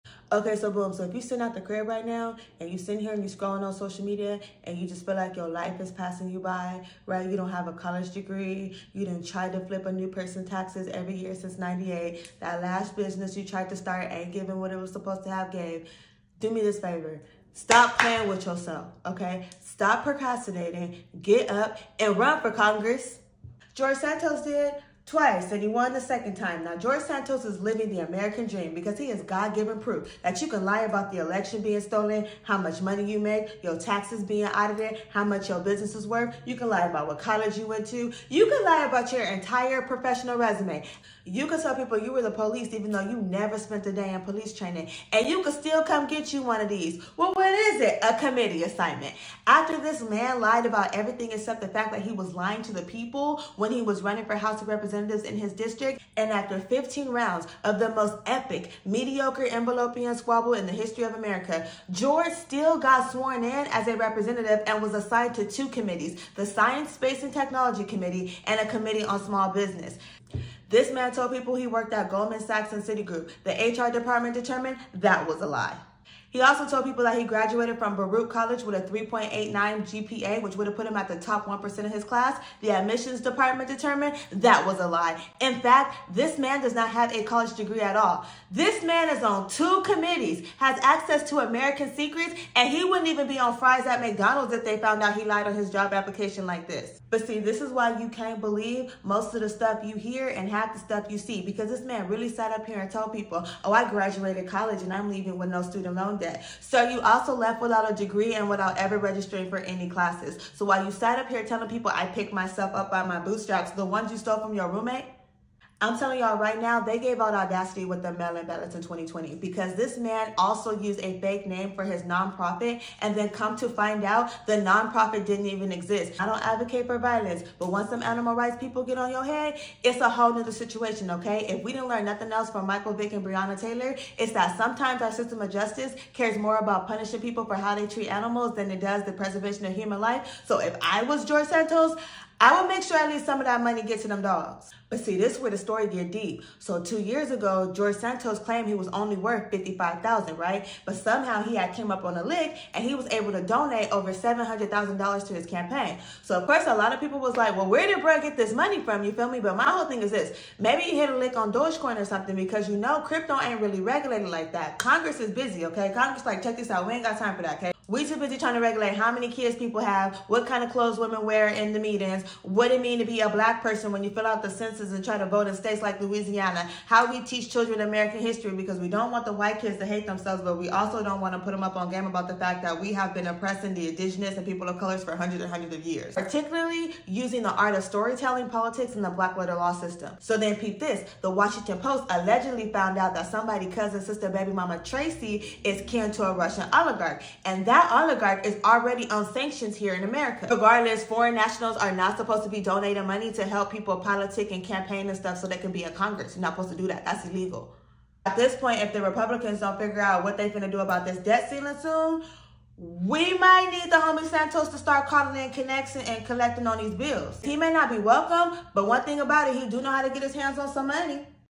George Santos placed on two House Committees despite recent accusations and ask that he resign
House Republican new comer, George Santos has been hit with a slew of allegations since he was elected. To date, there are claims that he lied about he professional experience, his ethnic origin, where he went to school and his net worth.

Support and Visit LegallyHype.com